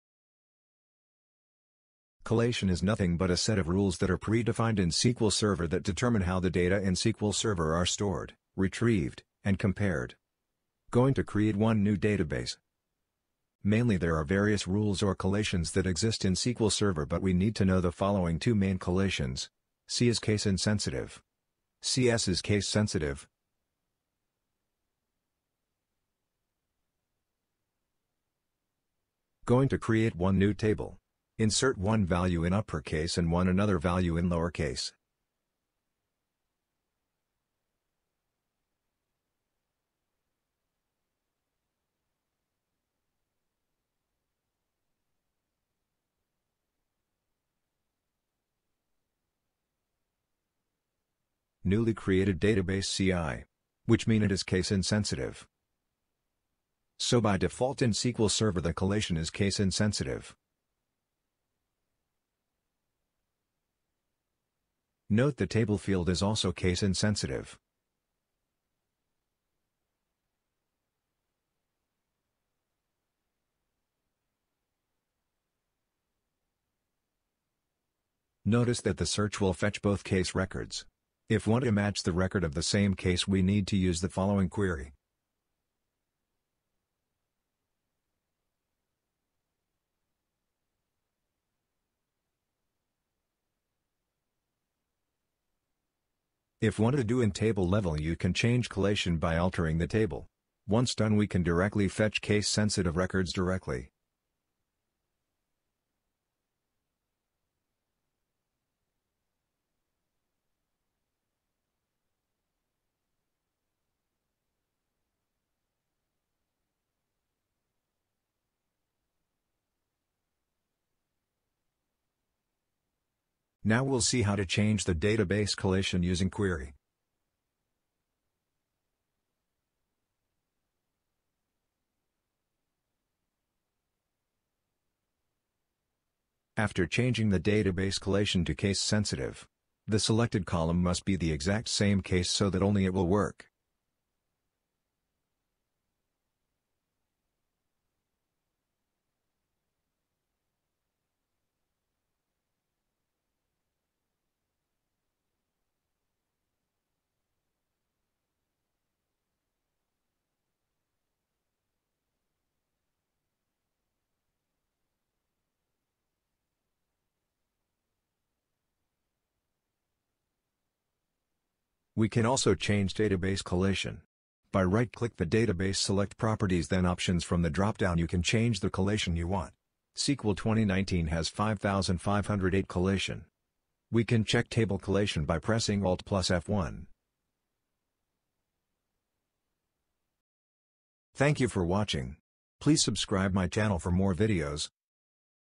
25.Collation in SQL Server 2019
in this video, we are going to see how  to change Column level collation and  Database collation

To Alter Column level collation :
ALTER TABLE COLLATION_TABLE
ALTER COLUMN NAME VARCHAR(100)
COLLATE Latin1_General_CI_AS

To Change Database level Collation :
ALTER DATABASE COLLATION_TABLE
   COLLATE SQL_Latin1_General_CP1_CS_AS